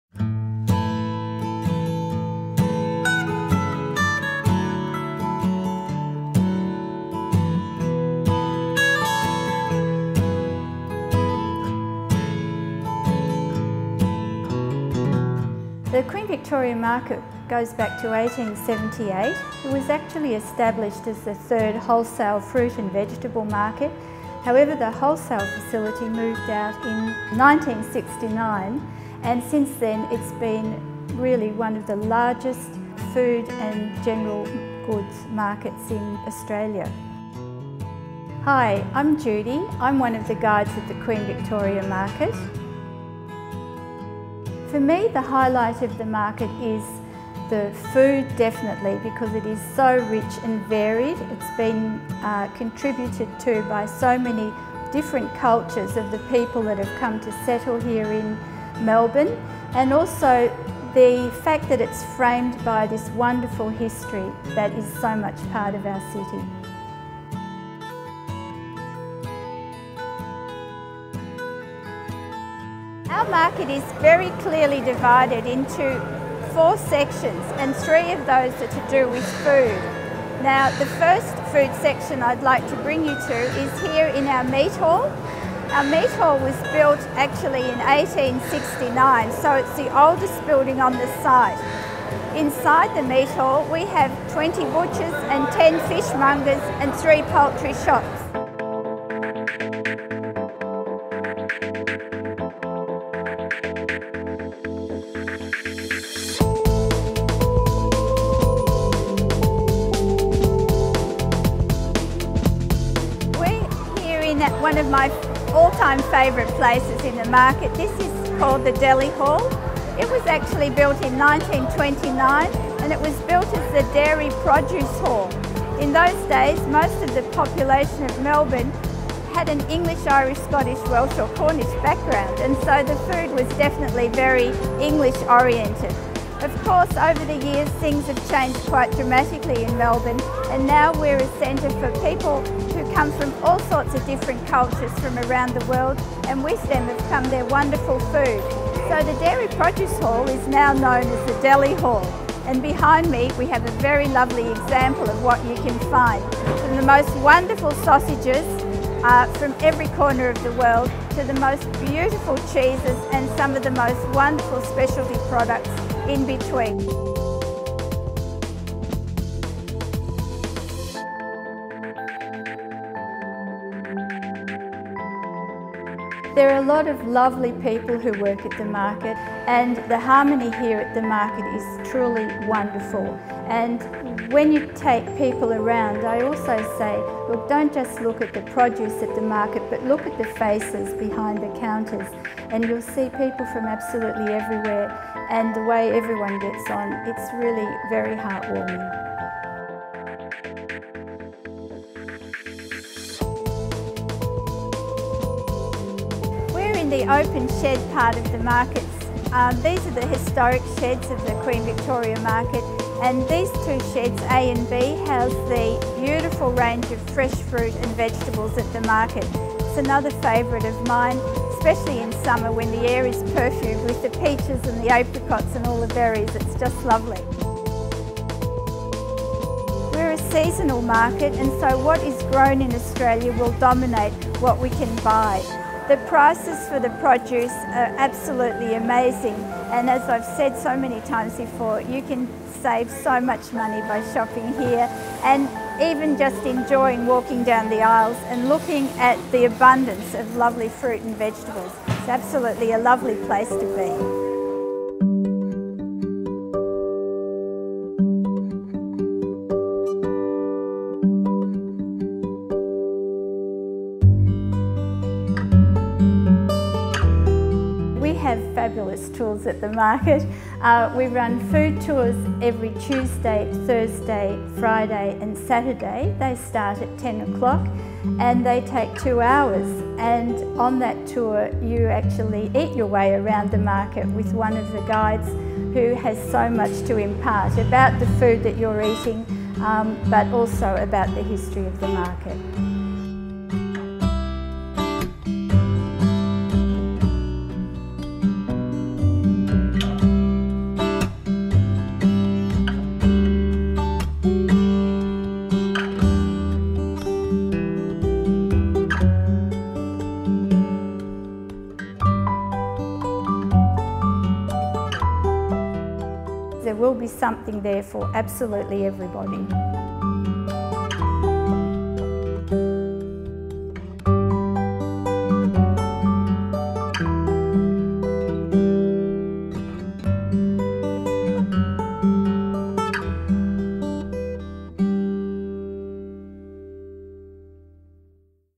QUEEN VICTORIA MARKET MELBOURNE
Take a tour of the iconic Queen Victoria Market in Melbourne.